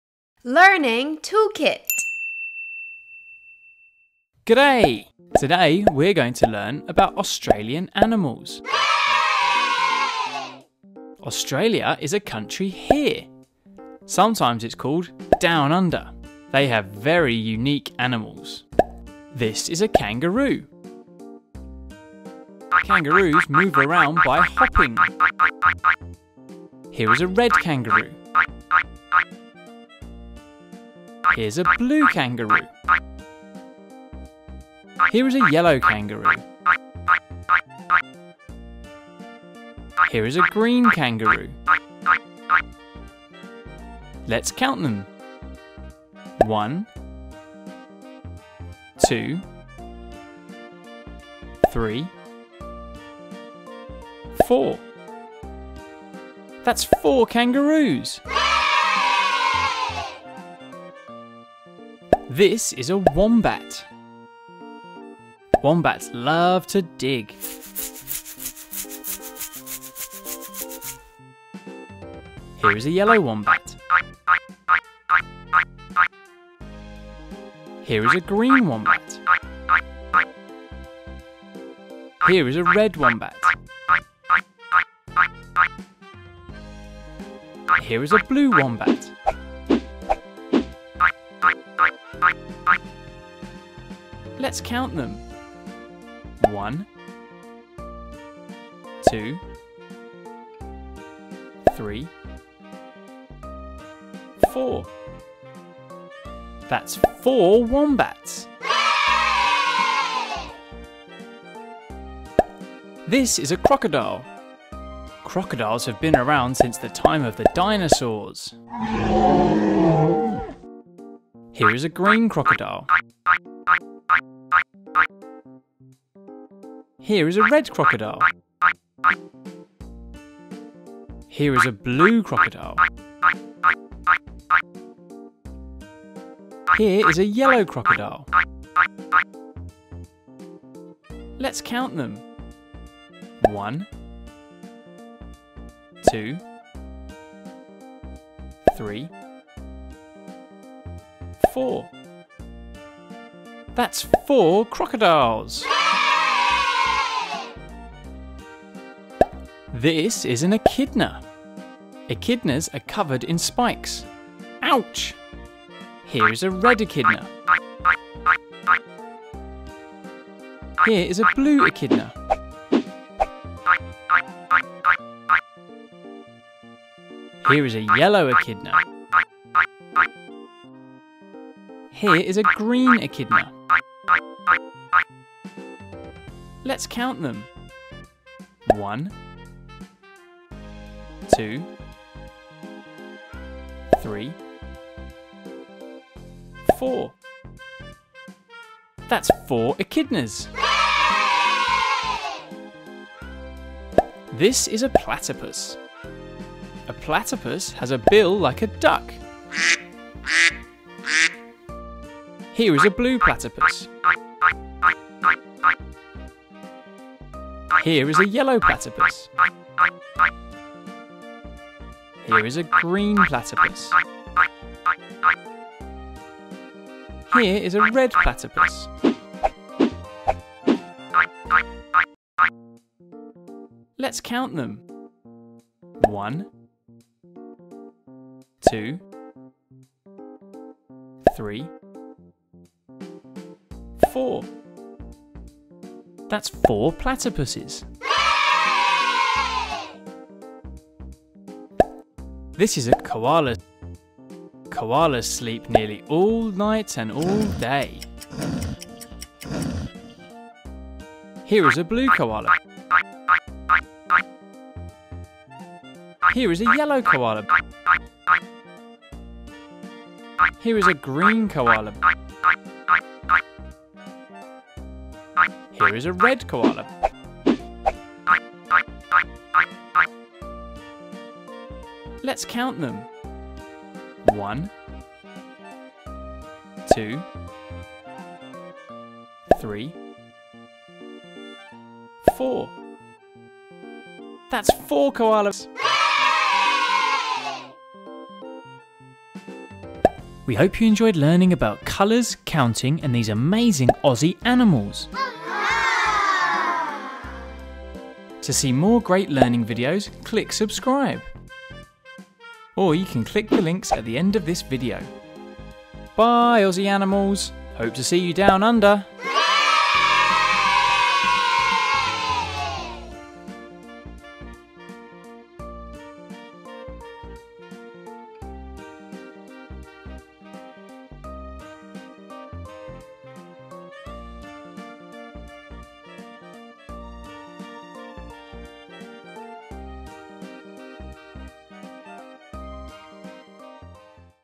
Learn COLORS & COUNTING WITH AUSTRALIAN ANIMALS | For Toddlers | From Learning Toolkit
Let's learn colors and counting with some amazing Australian animals! (Re-upload with corrected fact!)

Kangaroos!
Koalas!
Wombats!
Crocodiles!
Echidnas!
Platypuses!

High Quality, entertaining and educational videos.  For young learners - kids, pre-school, toddlers and babies.